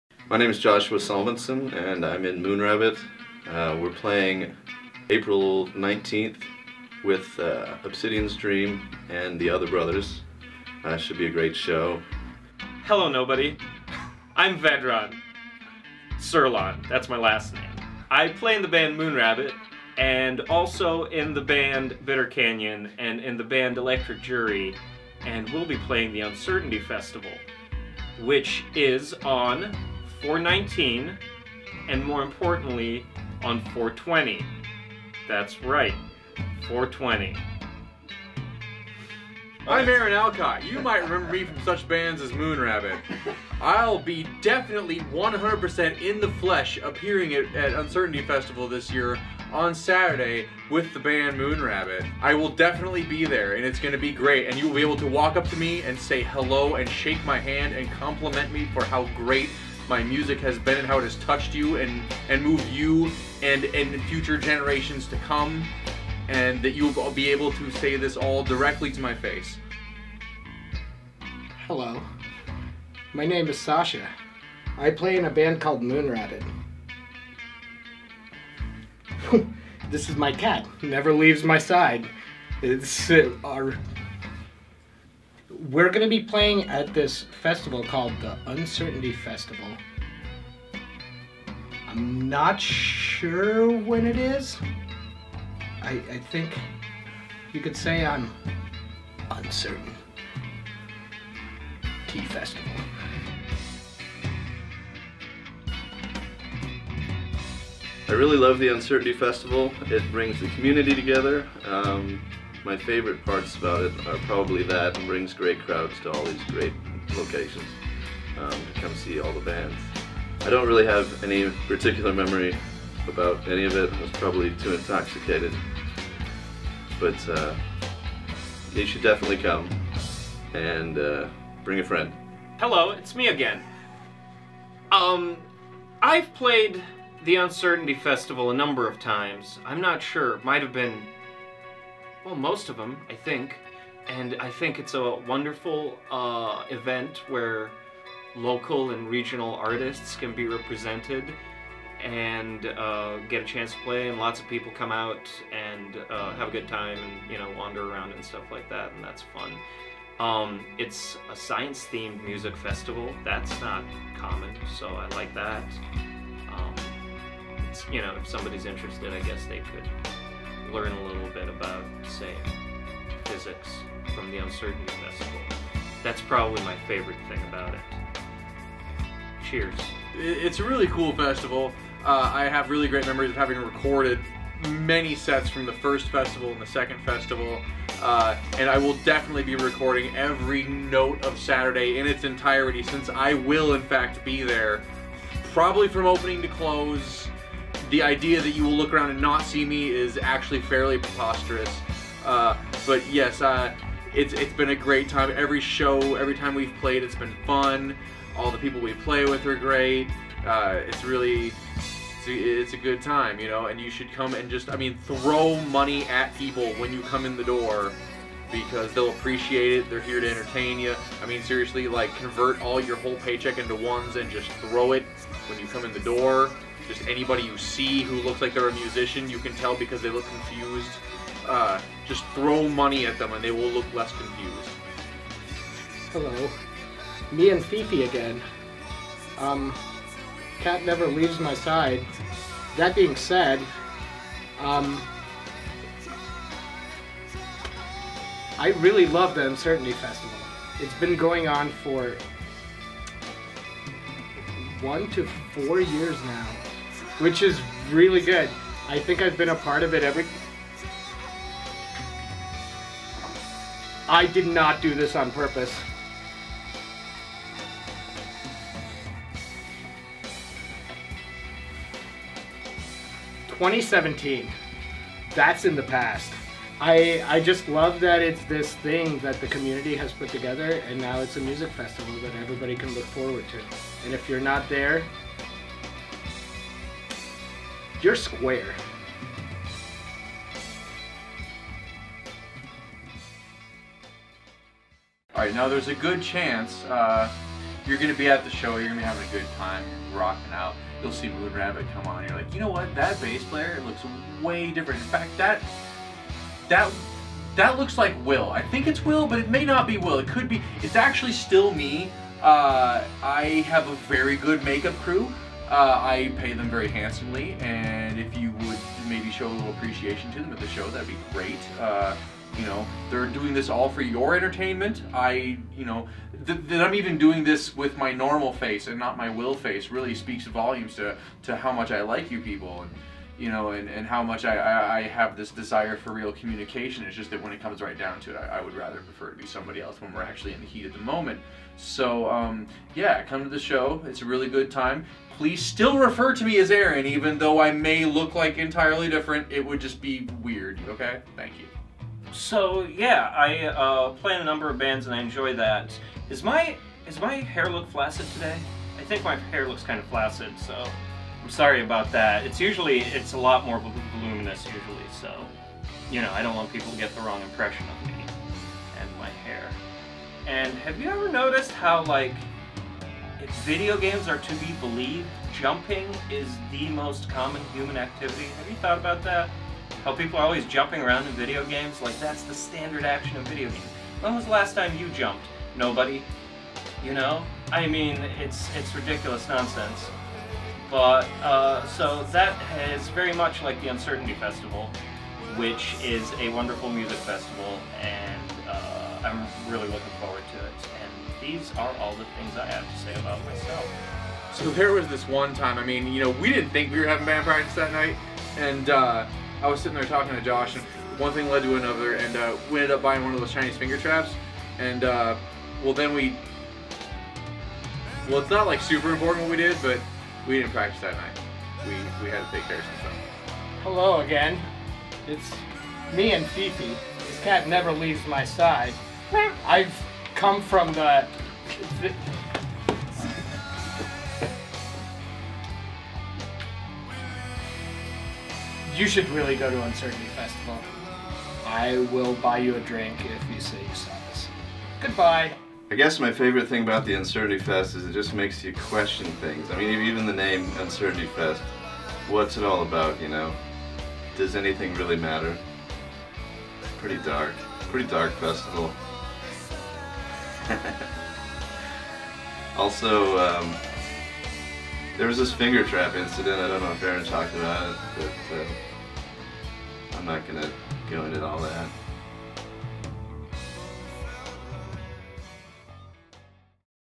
Uncertainty Festival 2019 - Moonrabbit
Moonrabbit talks about their upcoming performance at Uncertainty Festival 2019. And many other things.